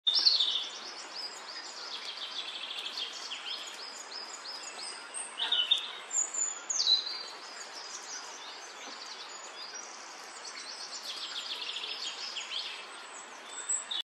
Feeding Pine Marten
A pine marten feeding at Queen Elizabeth Forest Park, in Loch Lomond and the Trossachs national park. Filmed on a Nikon Coolpix P1000 camera.